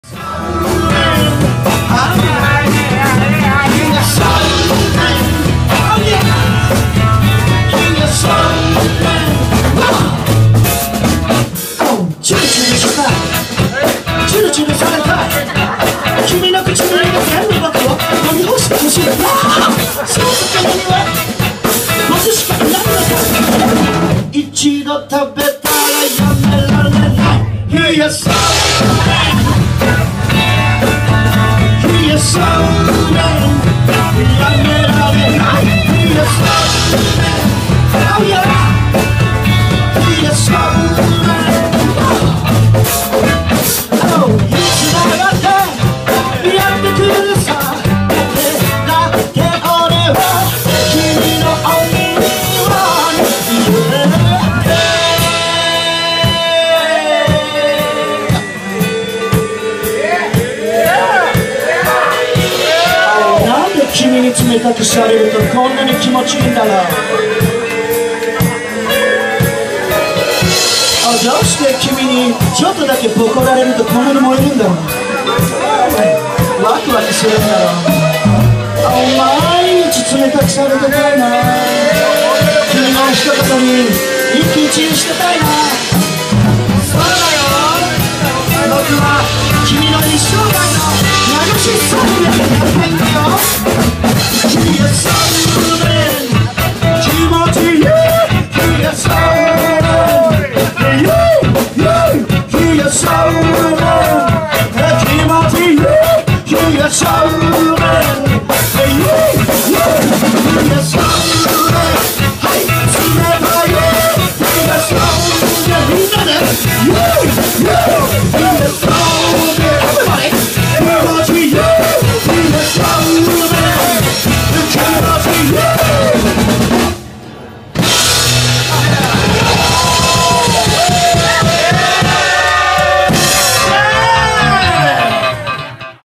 【潜入】モミーFUNK! ② @ Cafe&Bar LAST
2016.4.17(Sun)
『STOLEN MOMENTS』@ Cafe&Bar LAST

【Special Guest Live】
モミーFUNK!

【Live】
Clicker

【Dj】
TAKA-P a.k.a DISCOTARO
TAIKI